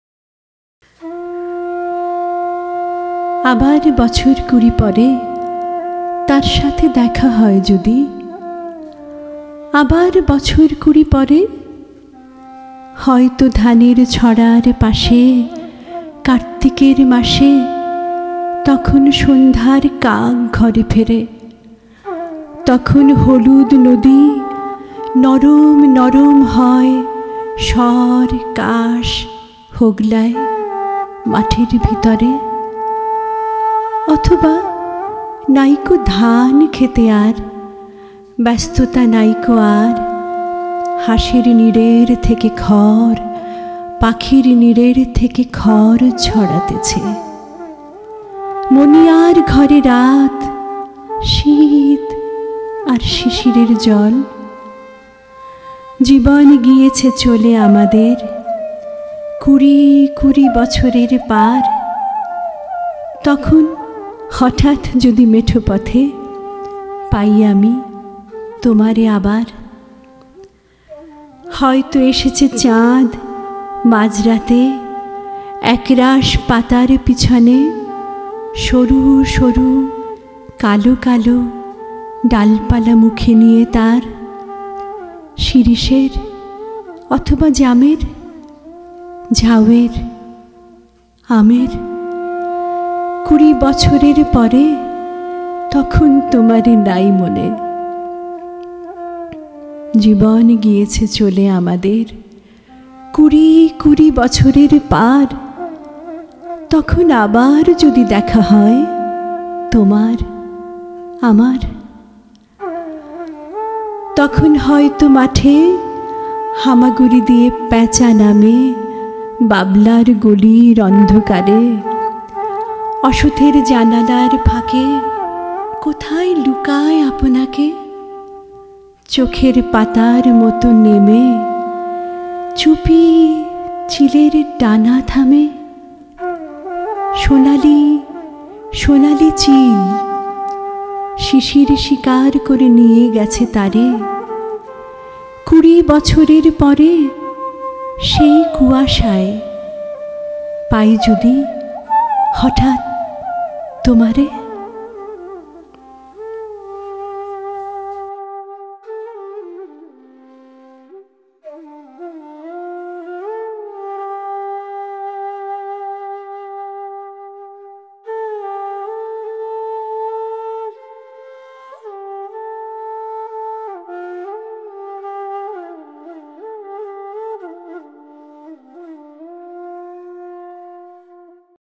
কুড়ি বছর পরে - জীবনানন্দ দাশ | আবৃত্তি - কেয়া কামাল | Kuri Bochor Pore- Jibonanondo Dash
Poem- Kuri Bochor Pore
Poet- Jibonanondo Dash
Recitation- Keya Kamal
Videography- Razib Hasan Russell
Post: Art Lab Media Productions